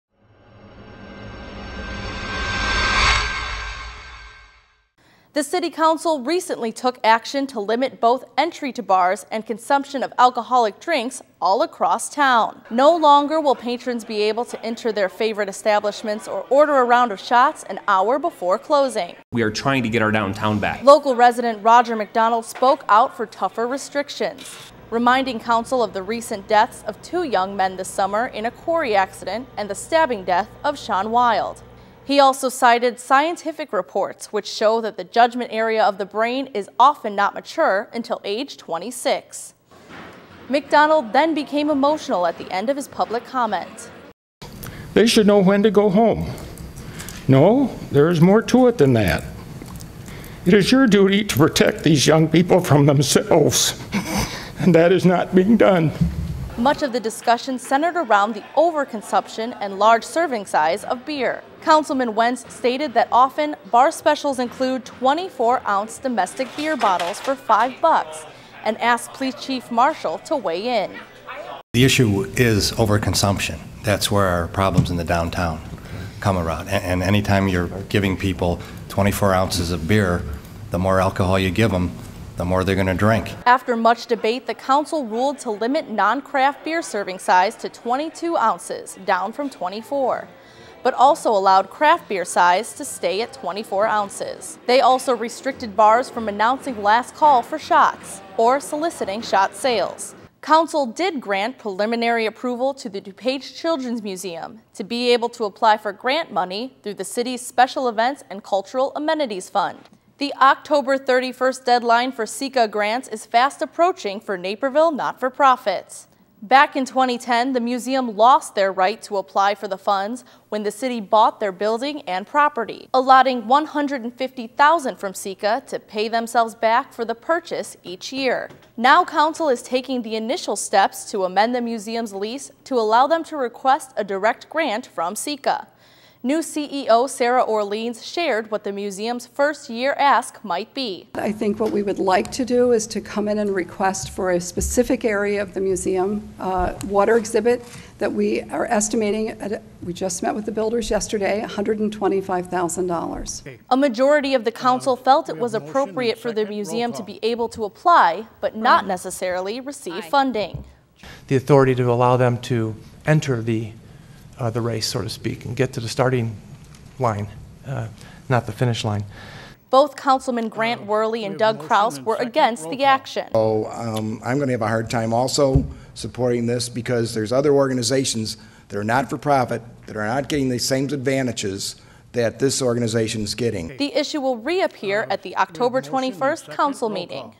Council Update
The City Council recently took action to limit both entry to bars and consumption of alcoholic drinks all across town. No longer will patrons be able to enter their favorite establishments or order a round of shots an hour before closing.

Councilman Grant Wehrli stated,  “We are trying to get our downtown back...”

Local resident, Roger McDonald spoke out for tougher restrictions, reminding council of the recent deaths of two young men this summer in a quarry accident and the stabbing death of Shaun Wild. He also cited scientific reports which show that the judgment area of the brain is often not mature until age 26.  McDonald then became emotional at the end of his public comment stating, “It is your duty to protect these young people from themselves and that is not being done…”

Much of the discussion centered on the over consumption and large serving size of beer.  Councilmen Dave Wentz stated that often, bar specials include 24 ounce domestic beer bottles for five bucks and asked Police Chief Marshall to weigh in. “The issue is over consumption. That’s where our problems in the downtown come from anytime you are giving people 24 ounces of beer, the more alcohol you give the more they’re going to drink,” concluded Marshall.

After much debate the council ruled to limit non-craft beer serving size to 22 ounces down from 24, but allowed craft beer size to stay at 24 ounces. They also restricted bars from announcing last call for shots, or soliciting shot sales.

Council did grant preliminary approval to allowing the DuPage Children’s Museum to be able to apply for grant money through the city’s special events and cultural amenities fund.

The October 31 deadline for SECA grants is fast approaching for Naperville not-for-profits. Back in 2010, the museum lost their right to apply for the funds, when the city bought their building and property, allotting $150,000 from SECA to pay themselves back for the purchase each year. Now council is taking the initial steps to amend the museum’s lease to allow them to request a direct grant from SECA.

New CEO Sarah Orleans said, “What we would like to do is come in and request for a specific area of the museum like Water Exhibit and we are estimating $125,000.”

A majority of the council felt it was appropriate for the museum to be able to apply but not necessarily receive funding. Councilman Steve Chirico said, ”If we don't feel like it fits then we can deny it. This is really just the authority to allow them to enter the race -- get to the starting line -- not to the finish line."

Both Councilman Grant Wehrli and Doug Krause were against the action. Councilman Doug Krause said, “It gives the museum an unfair advantage over other nonprofit organizations that apply for the events and culture funding.

The issue will re-appear at the October 21st council meeting.

Finally, Council decided to take a wait and see approach to the on-going puppy mill issue. Postponing any decision until June of 2015.
Advocates on both sides of the issue agreed to monitor the pending lawsuit in Cook County before making any decision.

Naperville News 17's Elizabeth Spencer Reports.